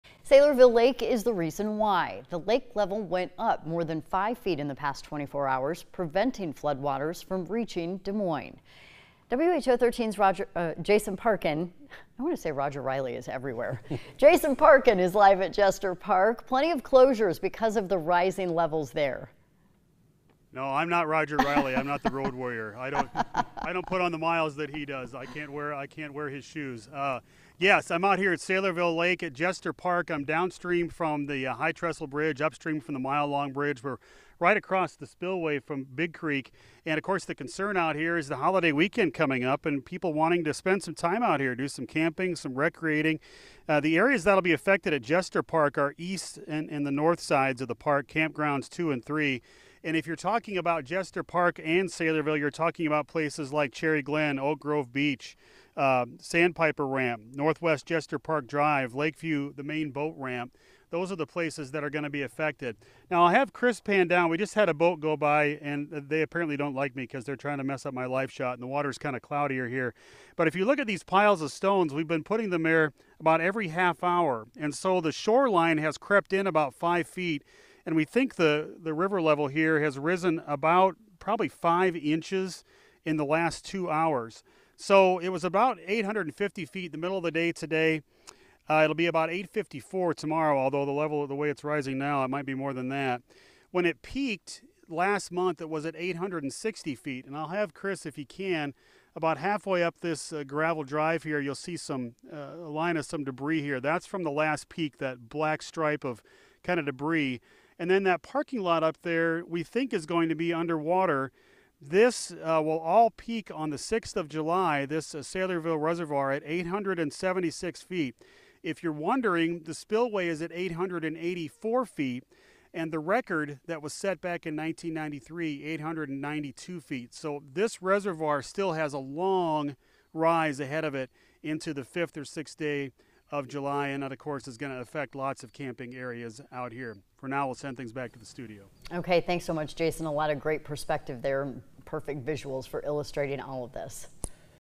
Closures at Jester Park due to rising water levels
WHO 13's Jason Parkin reports live from Jester Park to talk about all the closures there due to rising water levels.